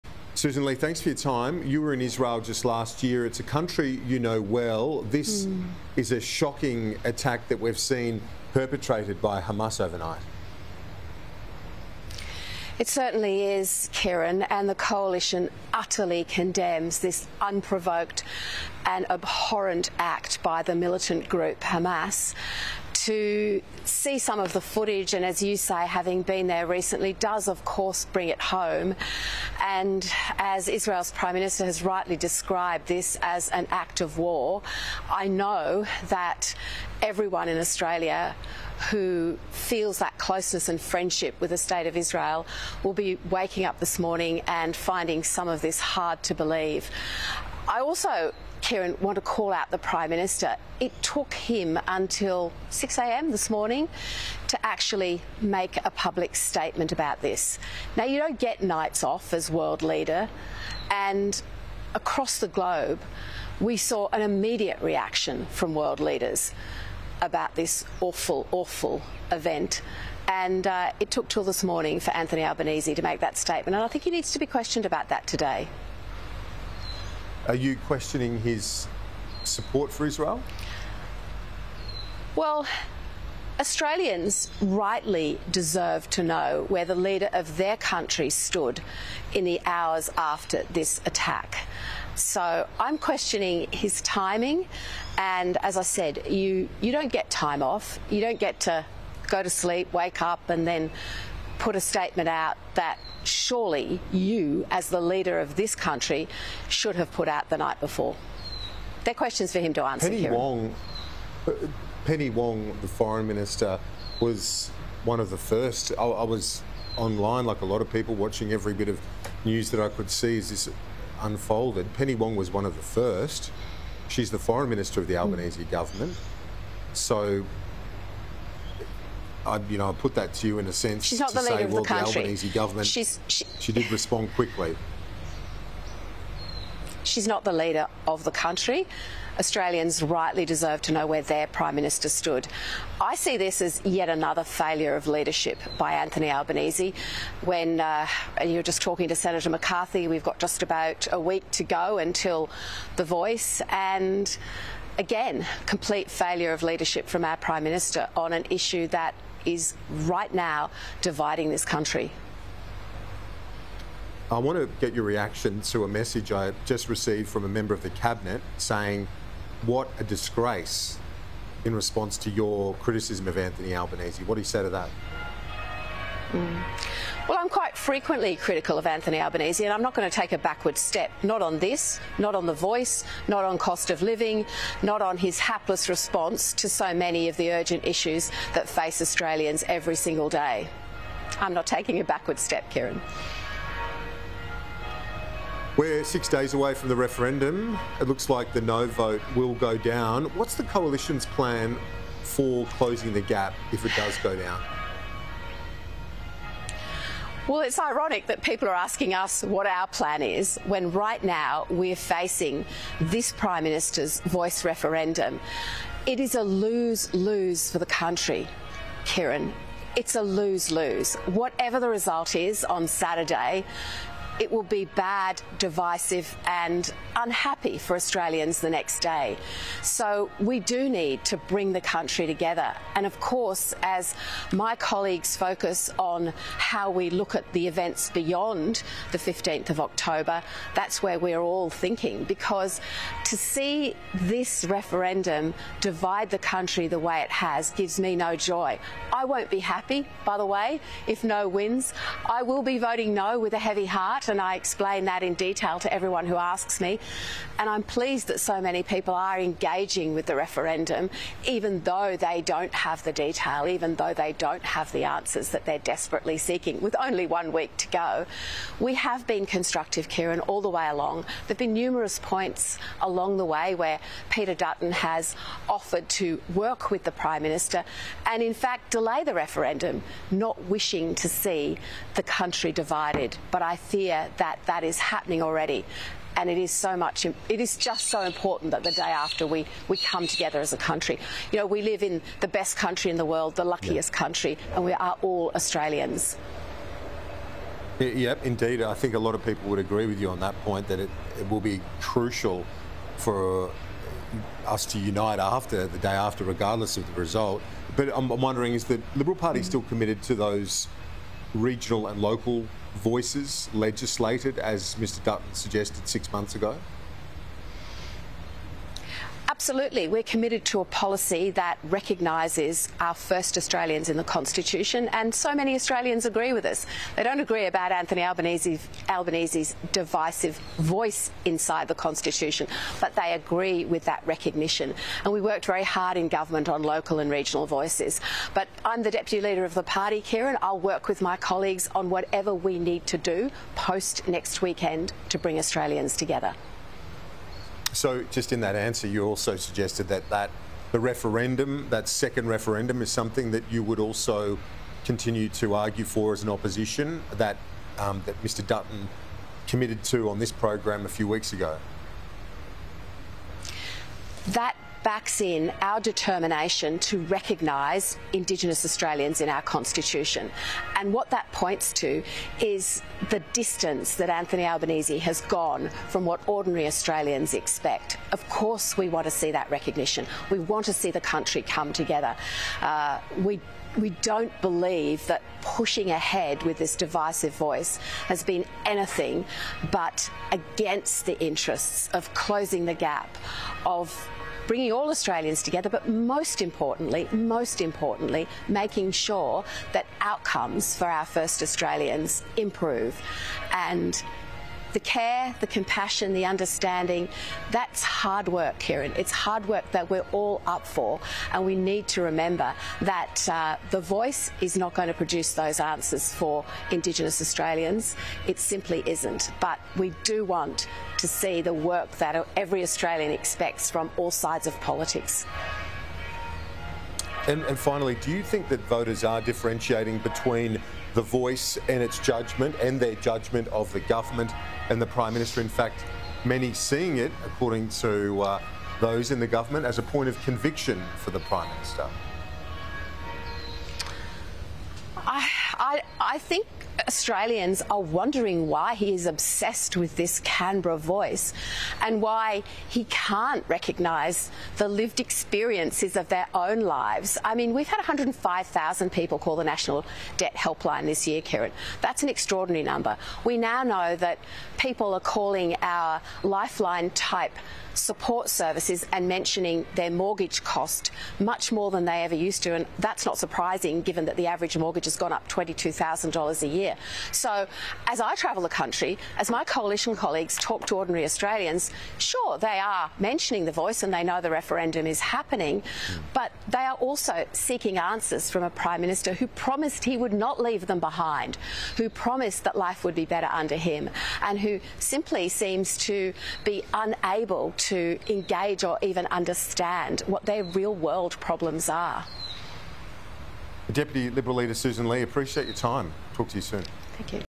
Coalition ‘utterly condemns’ Hamas' actions against Israel: Sussan Ley
Deputy Liberal Leader Sussan Ley says the Coalition “utterly condemns” militant group Hamas' actions against Israel.

A ‘state of alert for war’ has been declared in Israel.

“The Coalition utterly condemns this unprovoked and abhorrent act by the militant group Hamas,” Ms Ley told Sky News Australia.

“To see some of the footage ... does, of course, bring it home.

“As Israel’s Prime Minister has rightly described this as an act of war – I know that everyone in Australia who feels that closeness and friendship with the state of Israel will be waking up this morning and finding it hard to believe.”